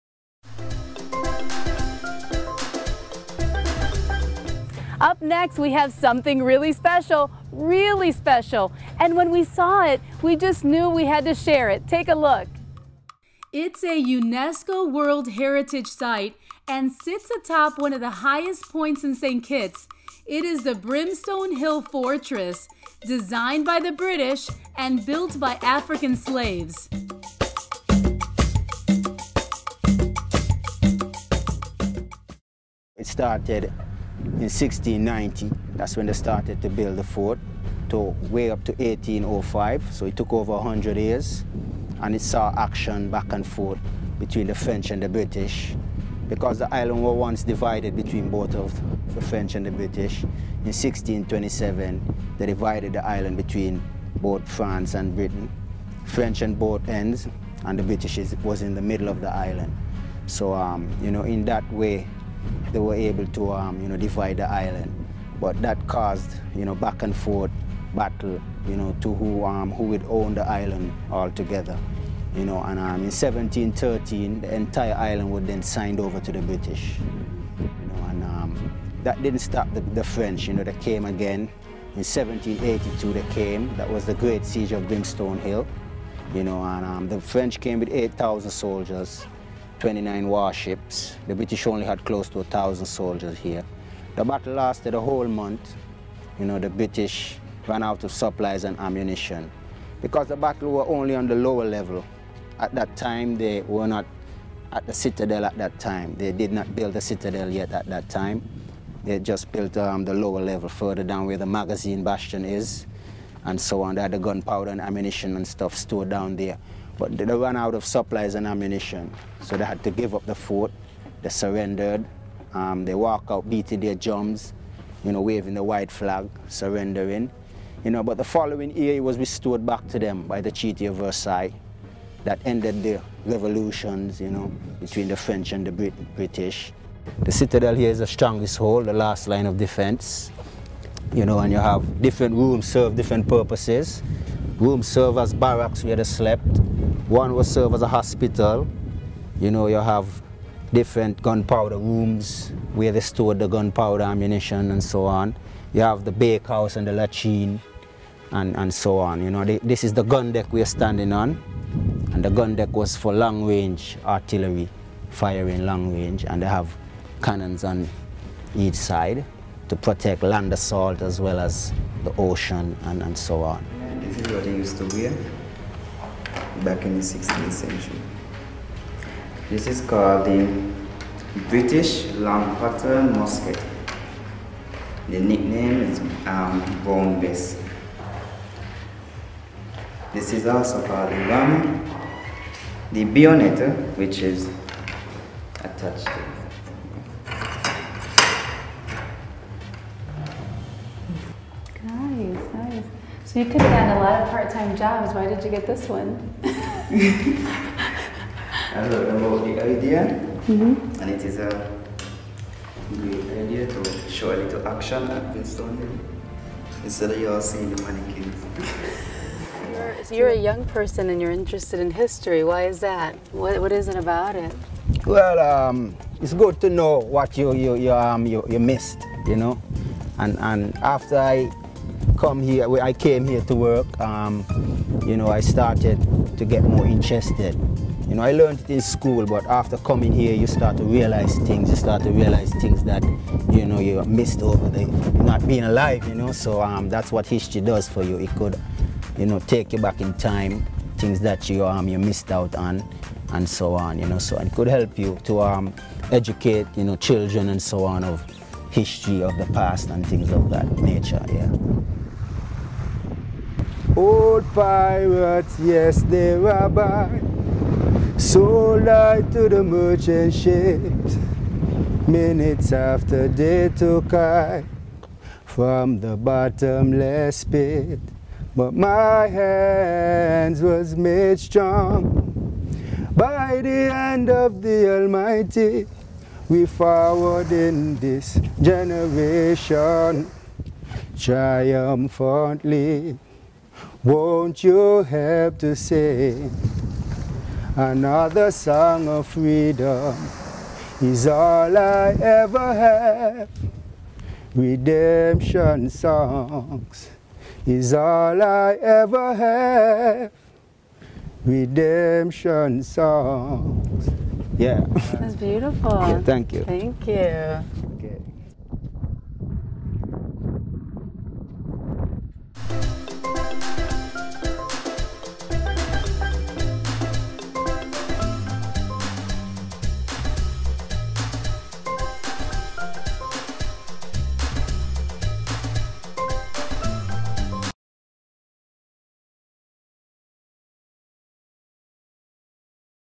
Brimstone Hill Fortress St Kitts
SOC visits the Brimstone Hill Fortress in St. Kitts, a World Heritage site.  Our guide shares how the history of Brimstone Hill has taught him to appreciate his own history and culture!